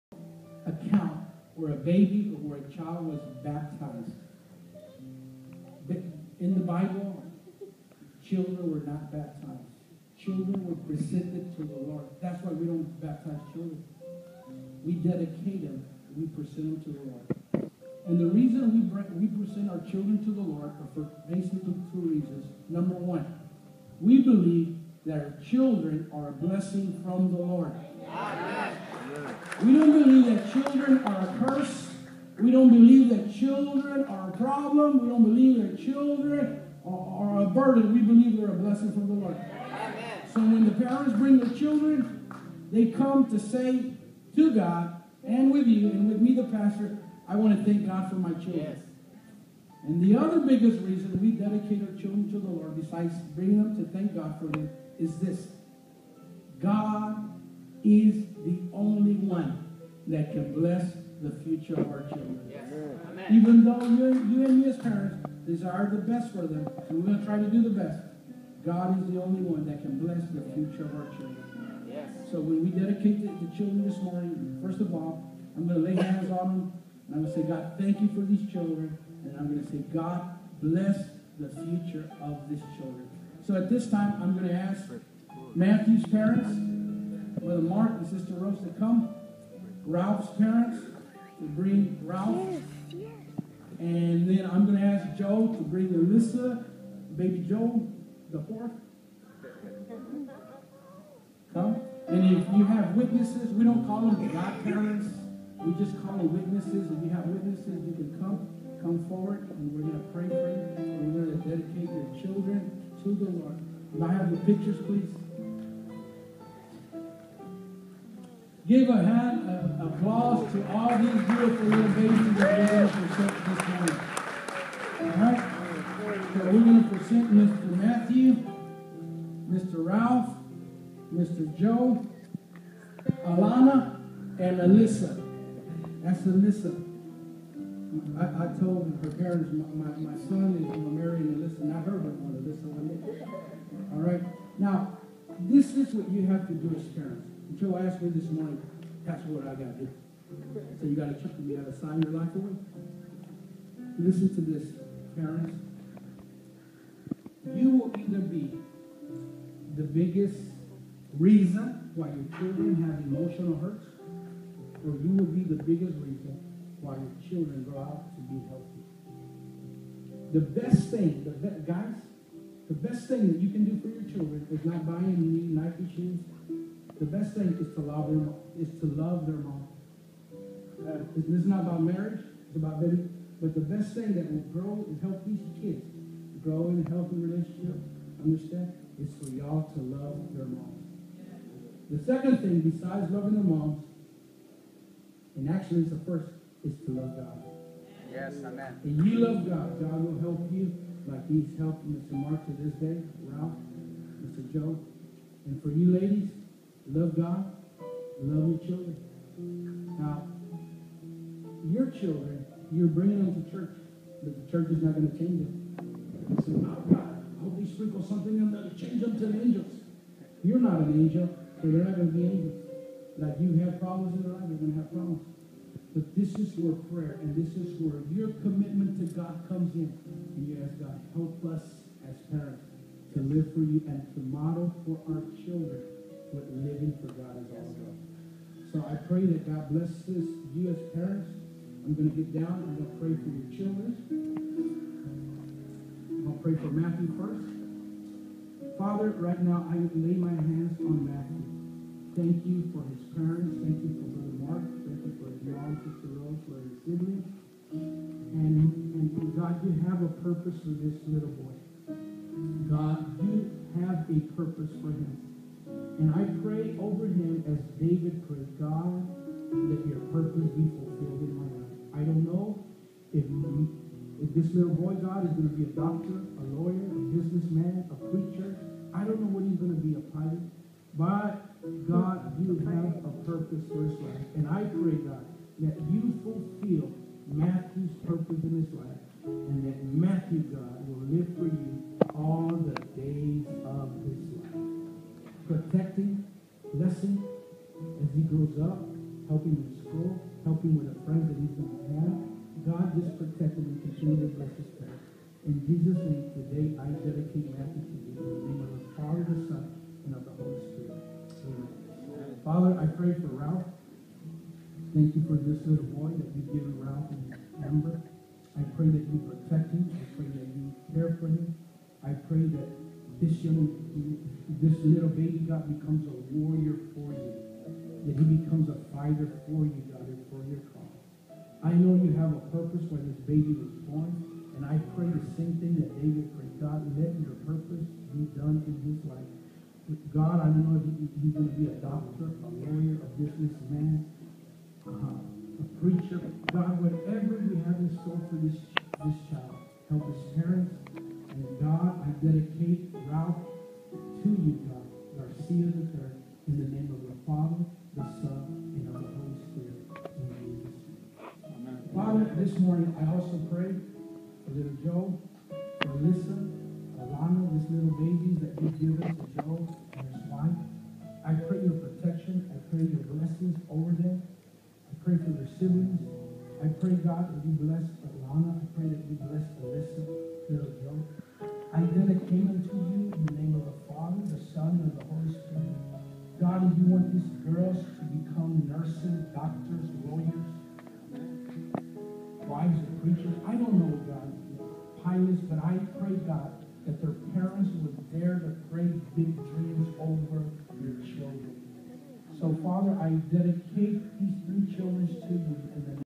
Baby dedication @ new life fellowship
Baby joe and Ralphie the plexin Texan giving our baby to the good lord